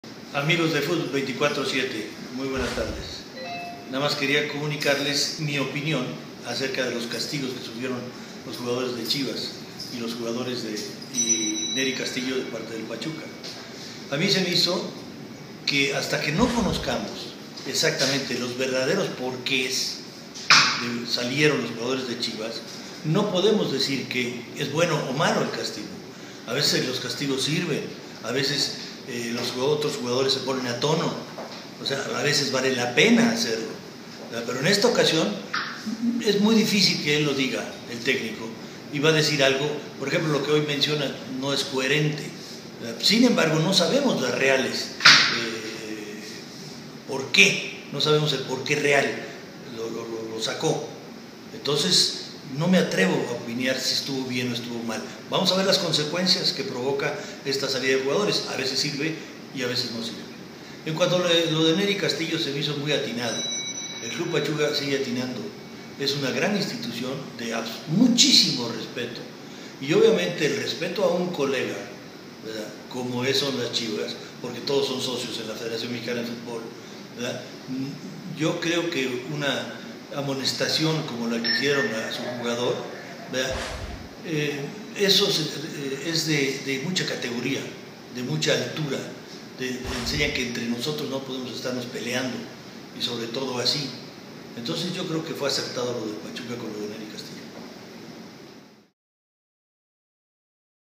VLOG MANUEL LAPUENTE - CASTIGO A JUGADORES
Manuel Lapuente nos da su opinión sobre los castigos hacia los jugadores de Chivas, así como la sanción que hizo el Club Pachuca hacia Nery Castillo por lo expresado en su cuenta de Twitter